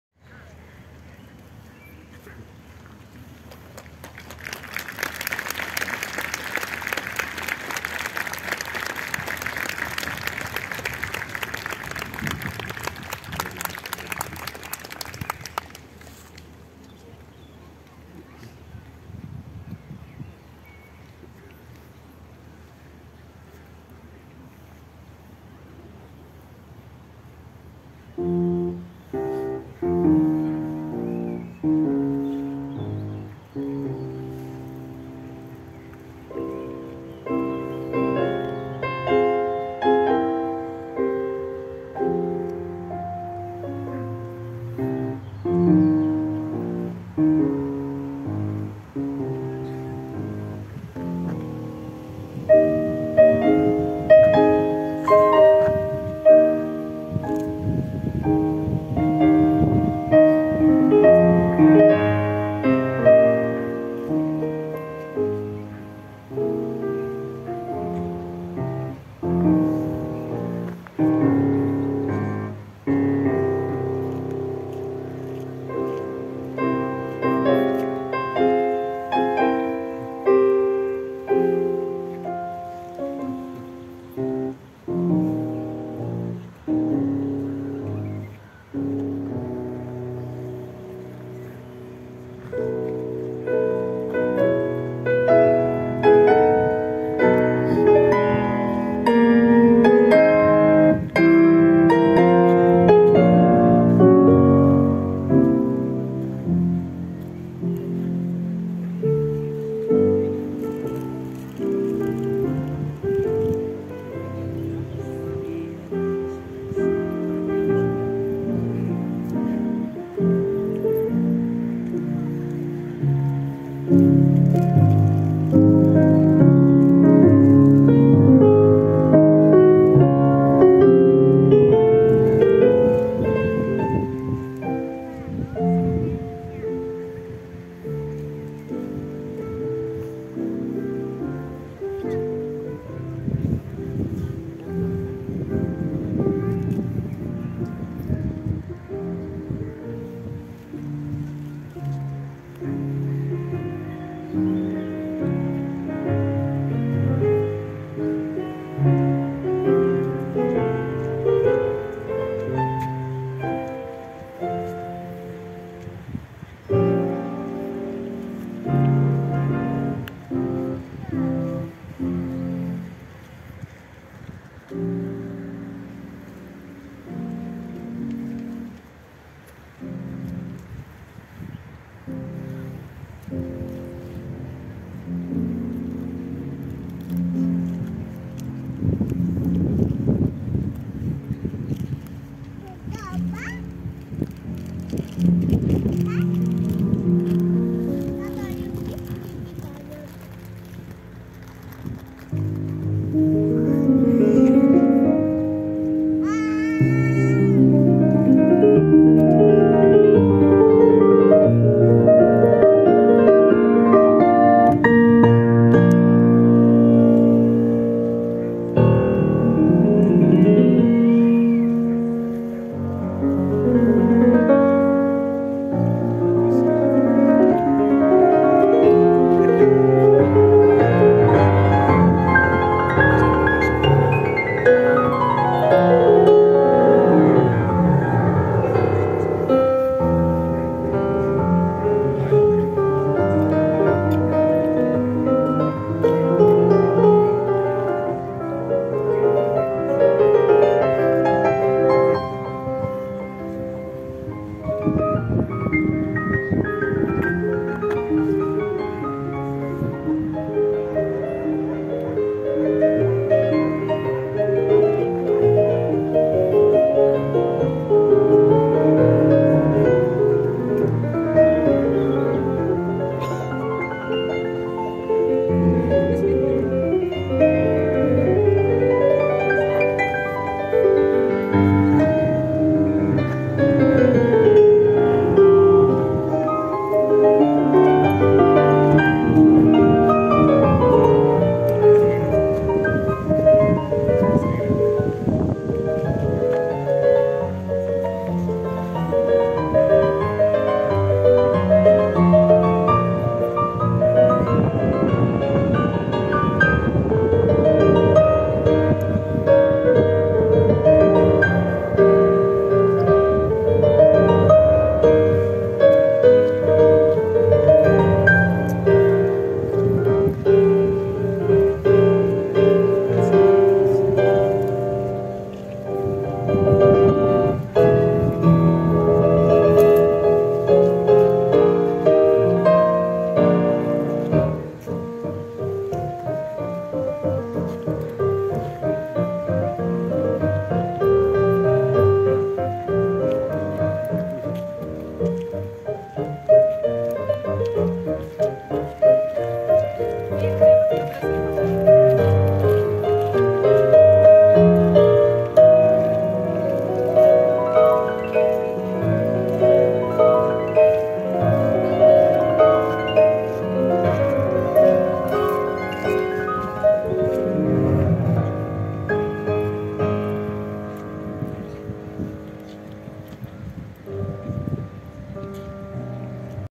Koncert Chopinowski w Warszawie (22.05.2022)
Koncert Chopinowski w Łazienkach. Gra Jakub Kuszlik (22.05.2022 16:05)